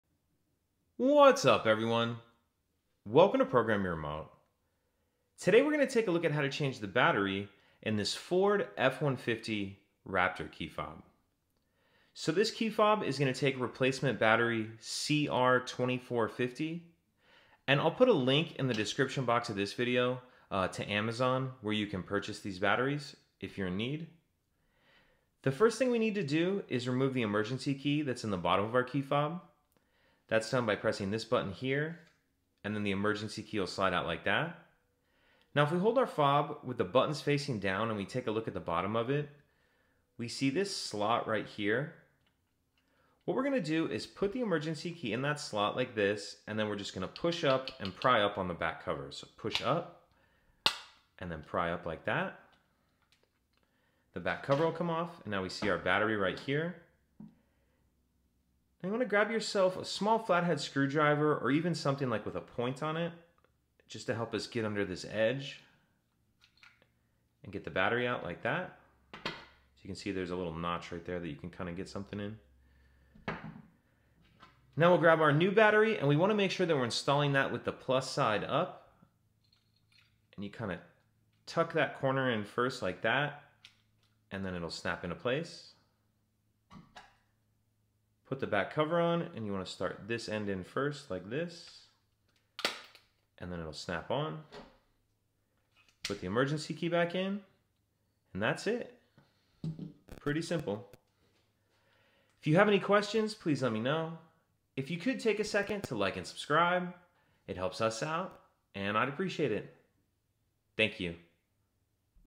2017 - 2022 Ford Raptor Key Fob Battery Replacement - How To Replace Change F150 Remote Batteries
A DIY video tutorial on how to change the remote fob key battery for a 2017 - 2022 Ford Raptor F-150.  You will need to open the Raptor remote key fob case In order to remove and replace the key fob battery.

This Ford Raptor F150 key fob uses a CR2450 replacement battery.

This DIY video tutorial will show you how to replace or change the replacement key fob battery in the following Ford models:
2017 Ford Raptor F150
2018 Ford Raptor F150
2019 Ford Raptor F150
2020 Ford Raptor F150
2021 Ford Raptor F150
2022 Ford Raptor F150